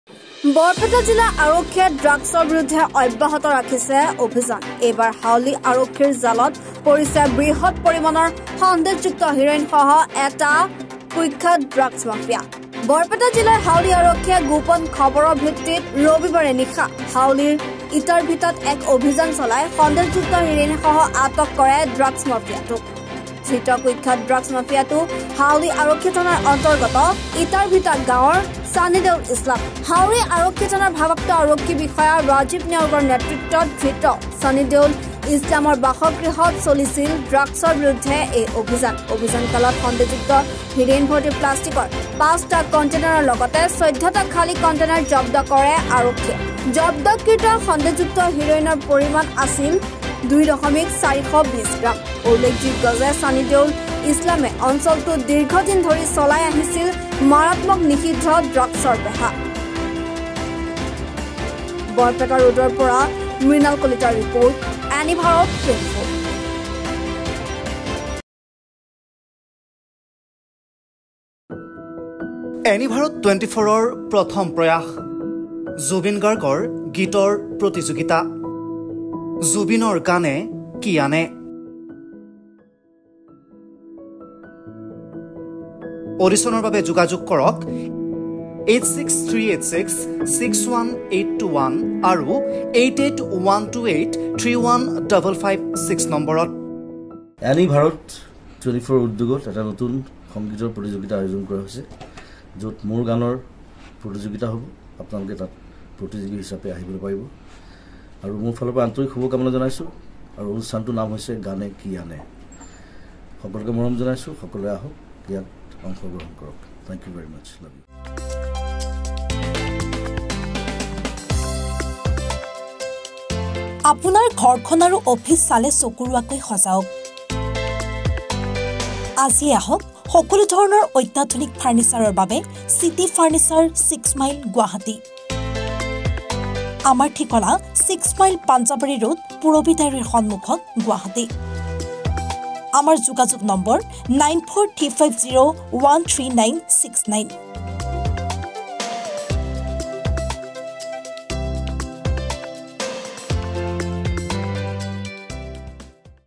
নিষিদ্ধ ড্ৰাগছৰ বিৰুদ্ধে হাউলী আৰক্ষীৰ বৃহৎ সফলতা
বুজন পৰিমাণৰ সন্দেহযুক্ত হেৰ'ইনসহ আটক এটা ড্ৰাগছ মাফিয়া